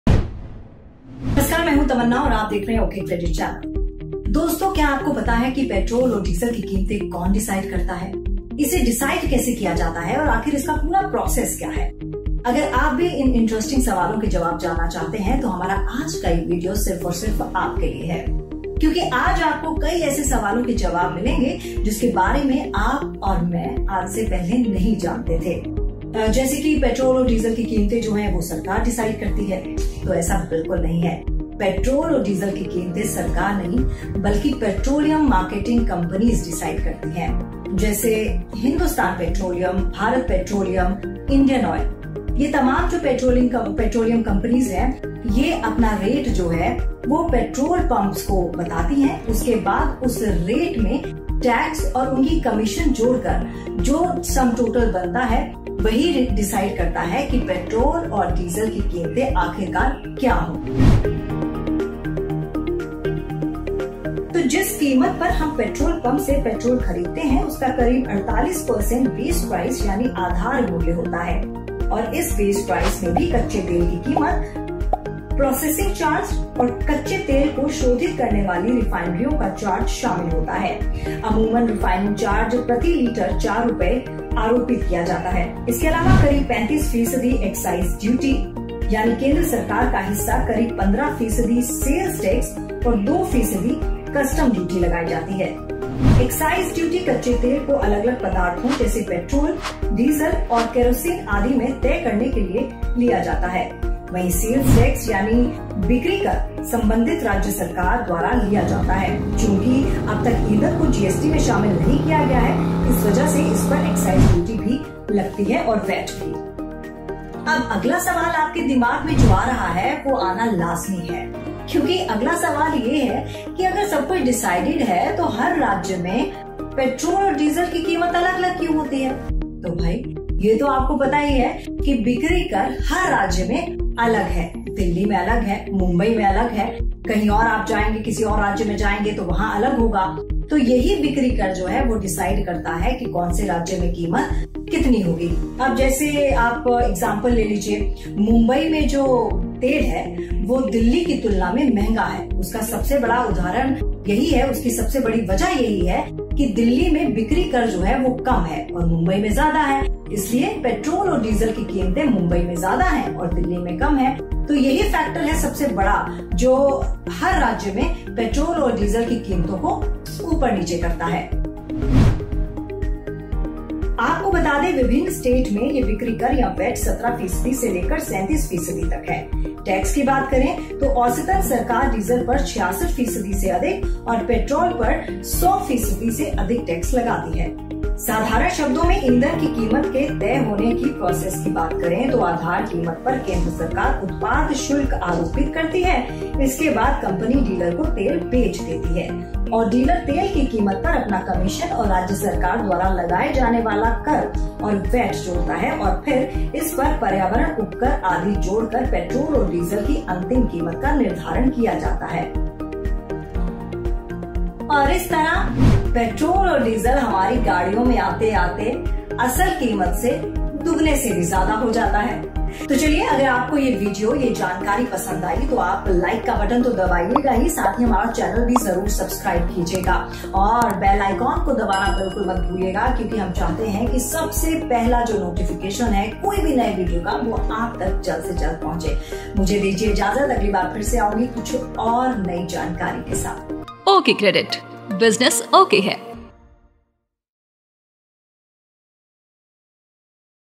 Petrol-Diesel की कीमत किस तरह decide होती है? | Expalined in HINDI | OkCredit
क्या आप जानते हैं पेट्रोल डीज़ल की कीमत किस तरह डिसाइड होती है, इसे करता कौन है और इसका प्रोसेस क्या है. अगर आप यह पूरा गणित समझना चाहते है तो यह वीडियो आपके लिए है. तो सबसे पहले तो आप यह जान लें कि भारत में पेट्रोल और डीजल की कीमतों पर सरकार का नियंत्रण नहीं है. अब बात आती है कि फिर कौन डिसाइड करता है ईंधन की कीमत तो आपको बता दें दोनों ईंधन की कीमतें आयल मार्केटिंग कंपनियां डिसाइड करती हैं. एक्चुअली पेट्रोल और डीजल के भाव जो हैं वह आयल मार्केटिंग कंपनियां जैसे कि इंडियन आयल, भारत पेट्रोलियम और हिंदुस्तान पेट्रोलियम द्वारा पेट्रोल पंप को दिए जाने वाले रेट के बाद टैक्स और उनका कमीशन जोड़ने के बाद बनने वाली रकम है. इस गणित को पूरी तरह समझने के लिए वीडियो को अंत तक देखें।